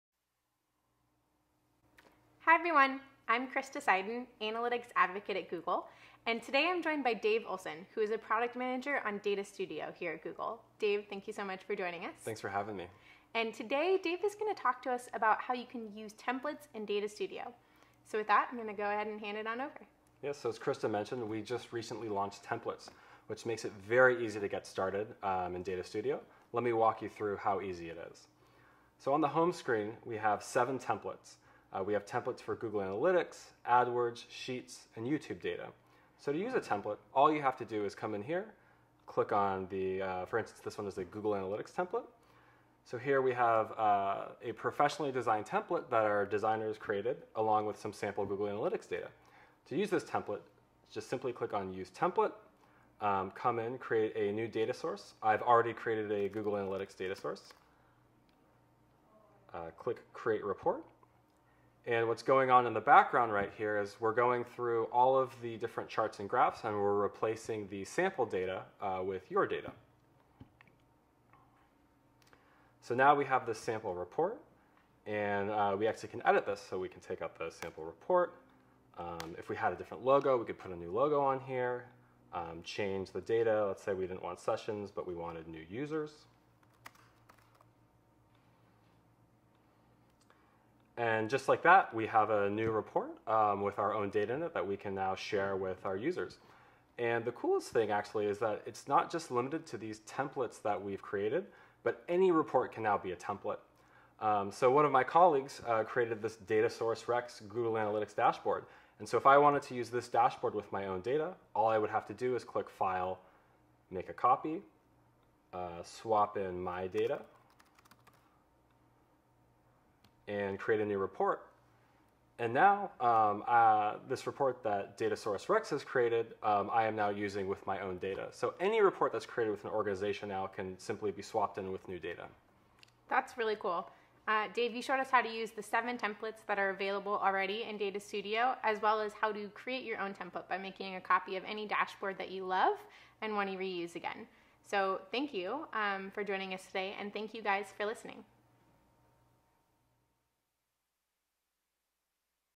Quick Tips: Using Data Studio Templates
In this Quick Tip, learn how to use templates in Data Studio to create new dashboards.